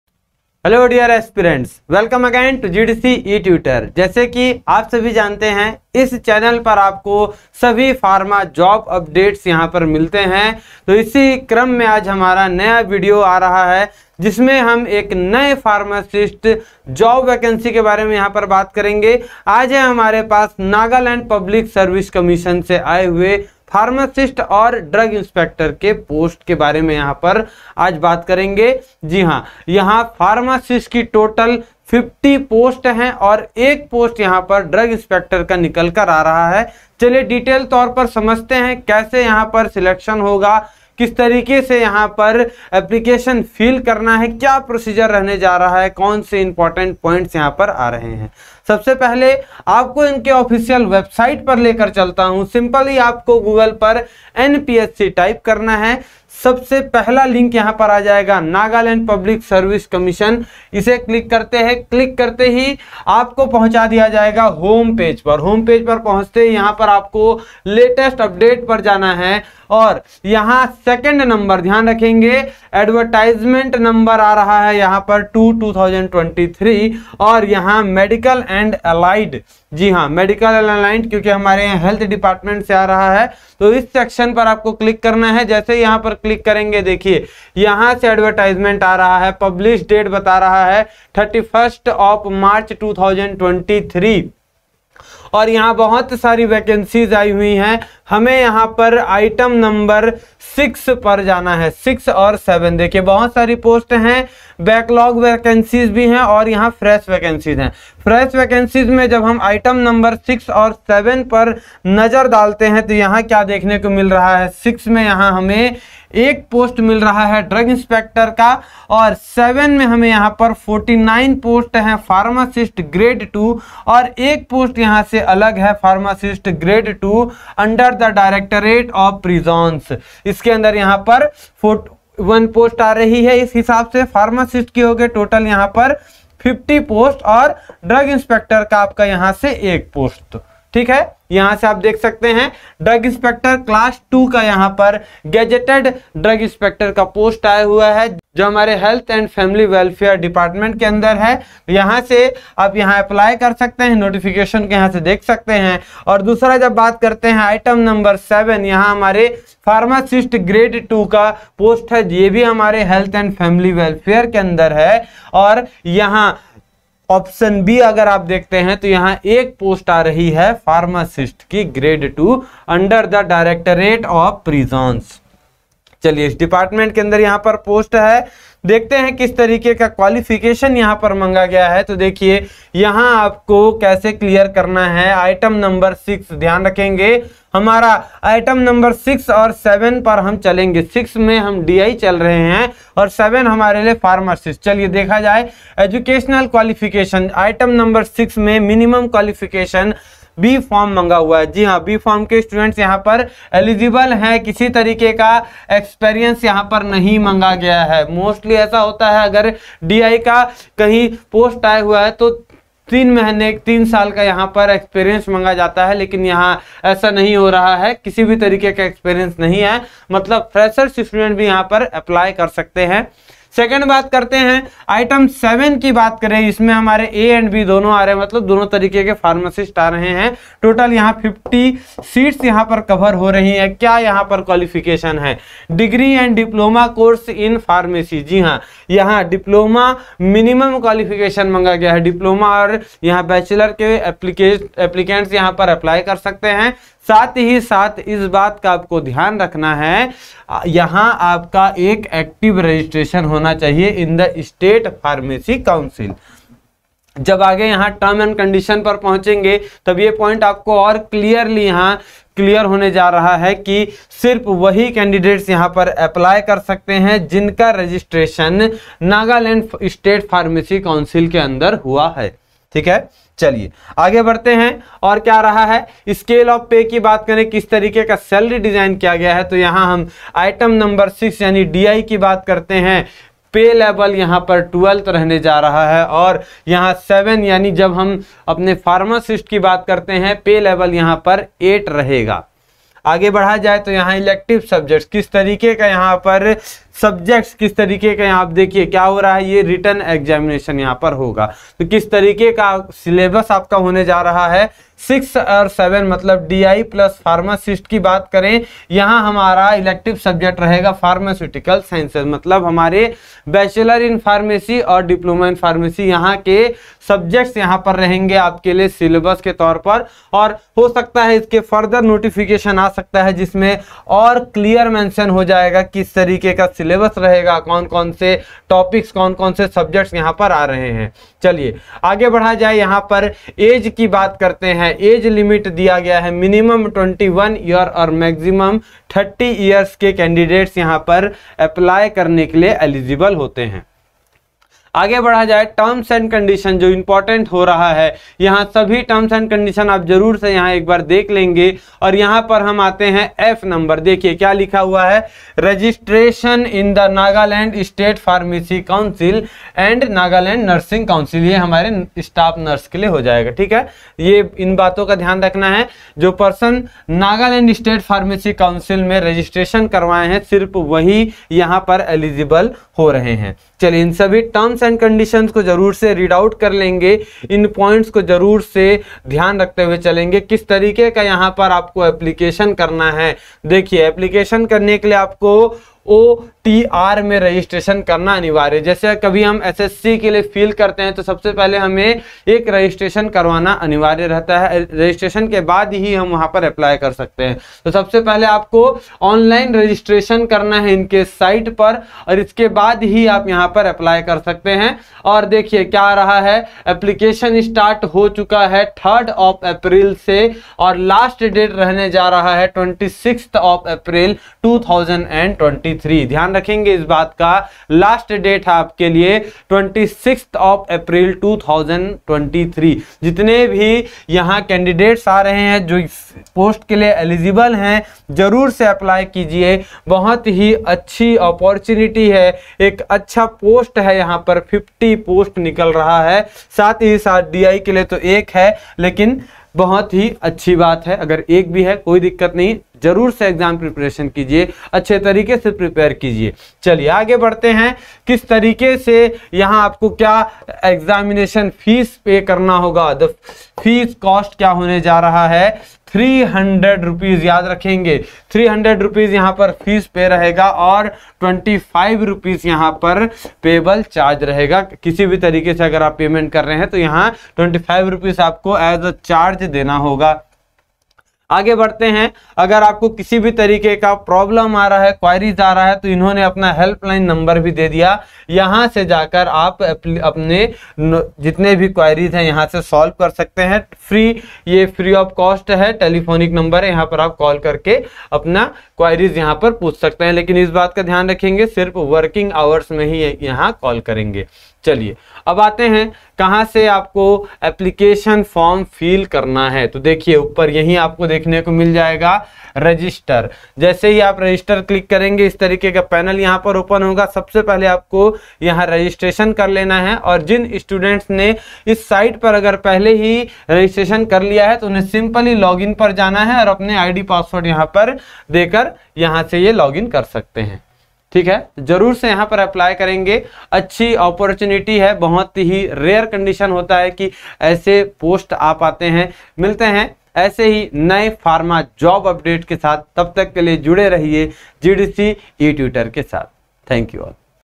NAGALAND PHARMACIST (50 POSTS) & DRUG INSPECTOR (01 POST) | COMPLETE NOTIFICATION | LAST DATE-26 APR
This video will be helpful for the candidates to clear all their doubts regarding NAGALAND state pharmacist & Drug Inspcetor job profile 2023
Where to fill form
How to fill the form
What is selection criteria
How much application fee
What is age relaxation
Who can apply
Other state candidates can apply
What is the exam pattern
Drug inspector eligibility
What is eligibility criteria

This video is helpful for all the pharmacy students willing to have a government job